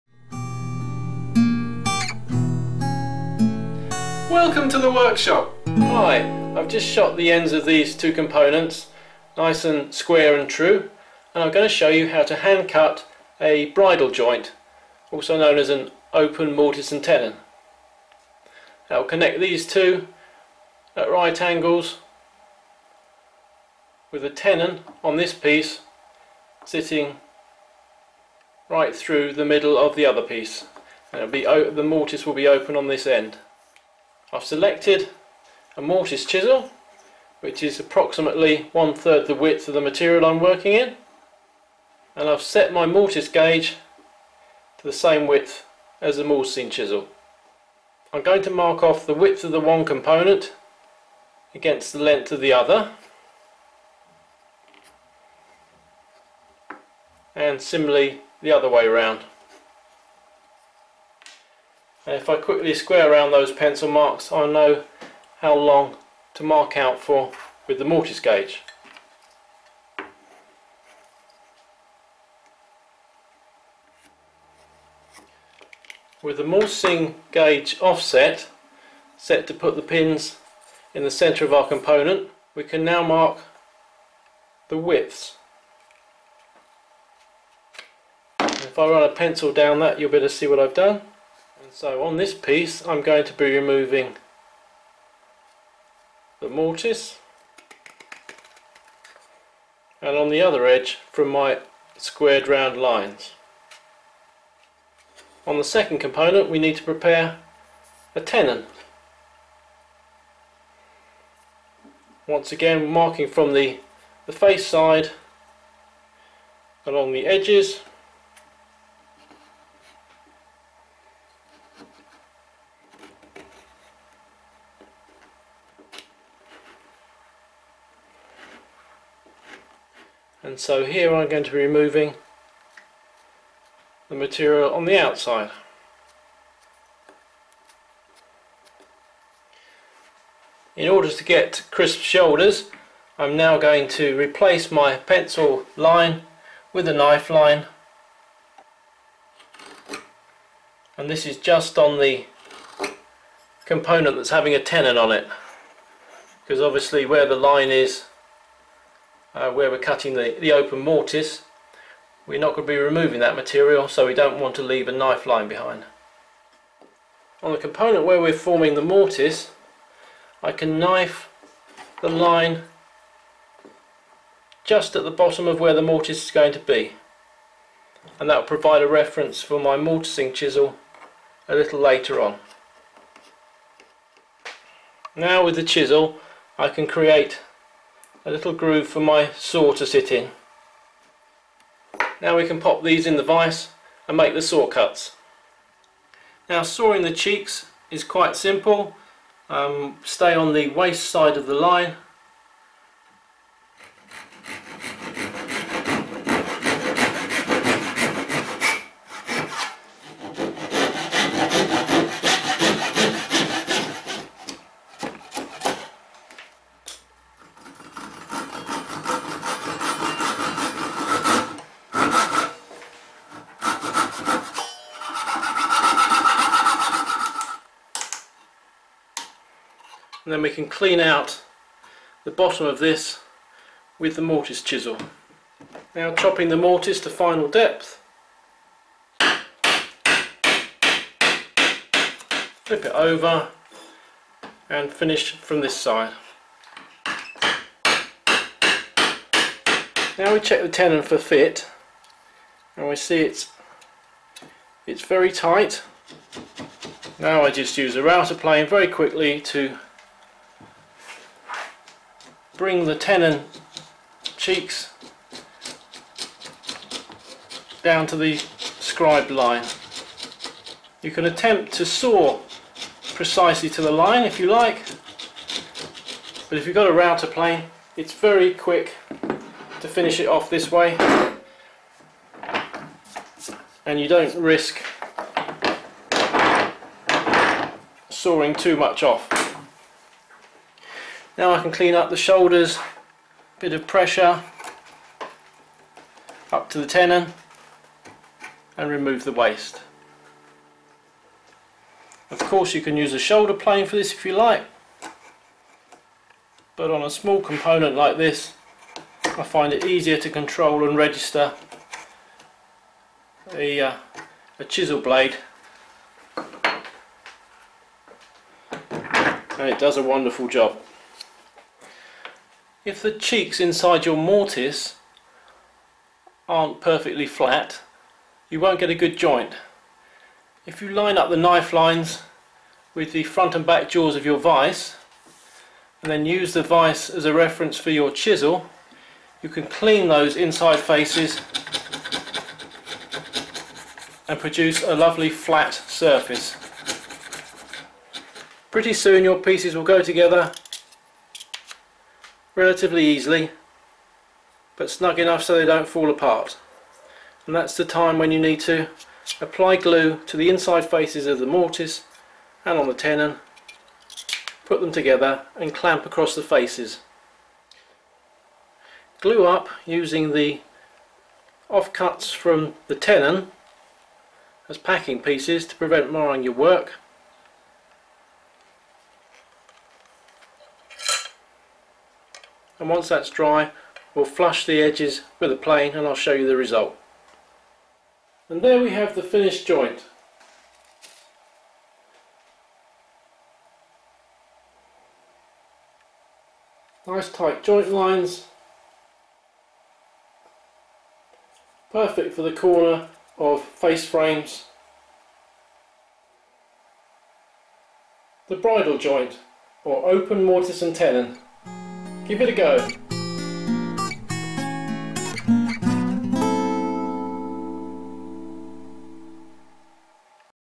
Hand Cut Bridle Joint or Open Mortise & Tenon
Watch as I demonstrate how to prepare a bridle joint, then try it yourself.

When forming a mortise and tenon joint at a corner, the usual method is to employ a haunch or secret haunch, so that the mortise does not extend right to the end of the component, but the tenon supports the full width of it's section to prevent cupping. However, it is also possible to create an open mortise with a full width tenon, known as a bridle joint, which replaces a lot of the effort of mortise chopping with two straight saw cuts.

The bridal joint is also ideal for quickly forming a tee intersection, where the open mortise straddles an 'enclosed' tenon - but that's another video...